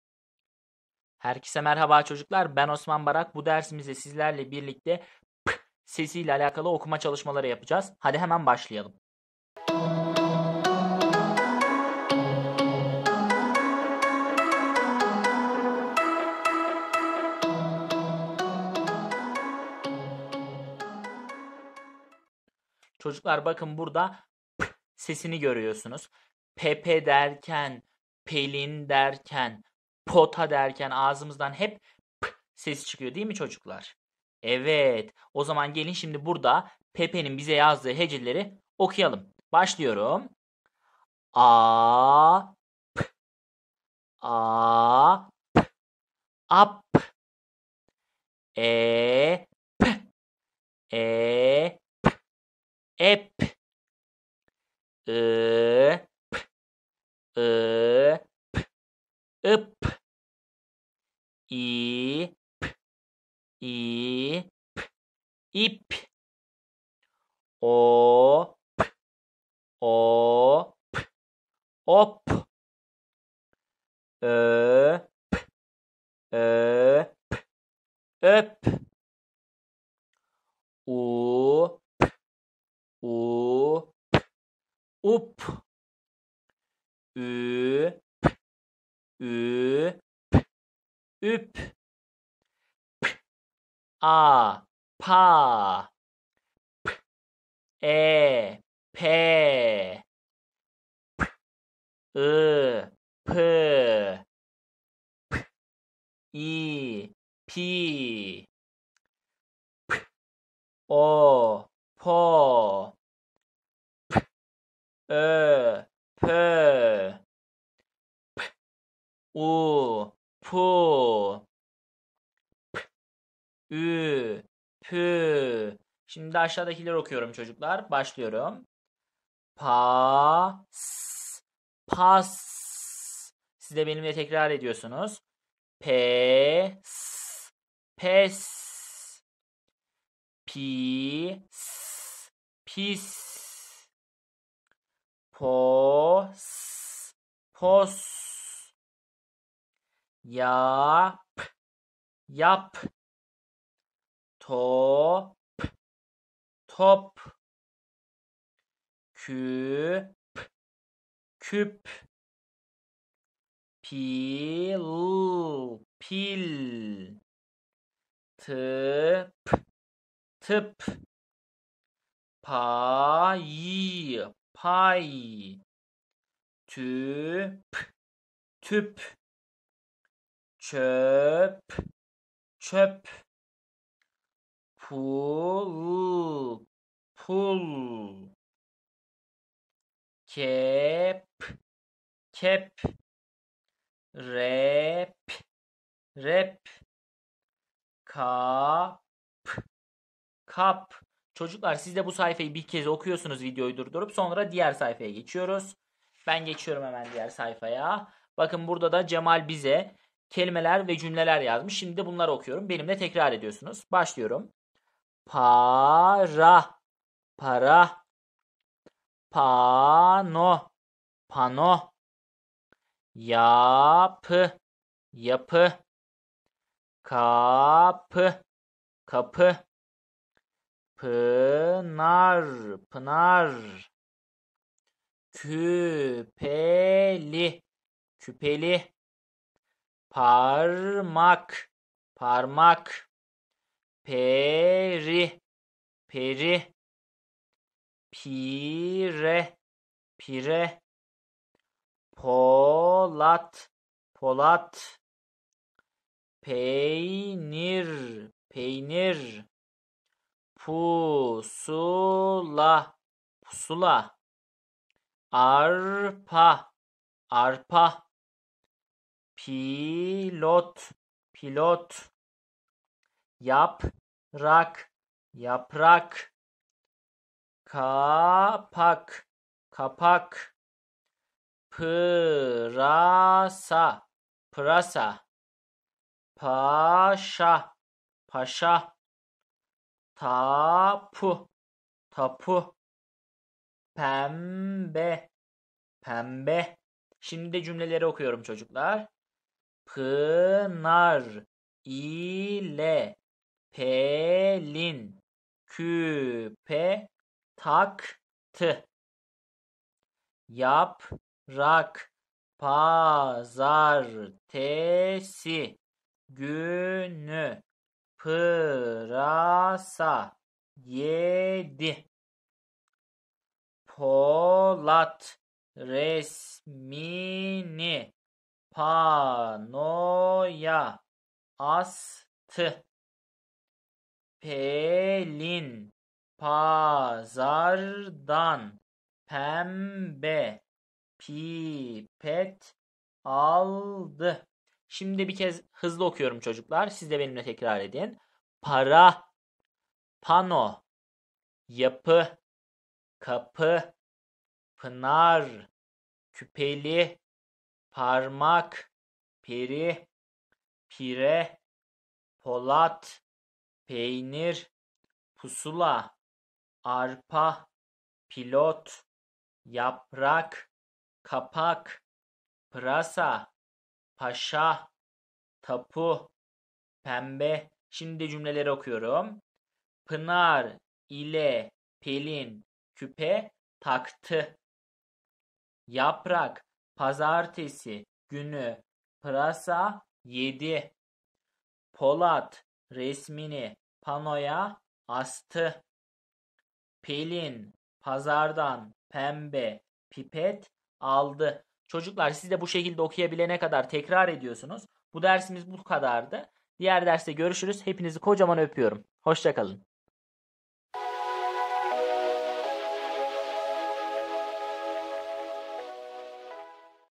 P Sesi Birleştirme Etkinlikleri Z,Ç,G,Ş,C,P | Hece, Kelime ve Cümle Oluşturma ☺️ (zçgşcp)
Bu dersimizde p sesi ile alakalı birleştirme çalışmalarına ve okuma etkinliklerine yer verdim. İyi dersler :)

Herkese merhaba kanalımda öğretmen adayları için bilgilendirici videolar ve ilkokul öğrencileri için ders anlatım videoları hazırlıyorum.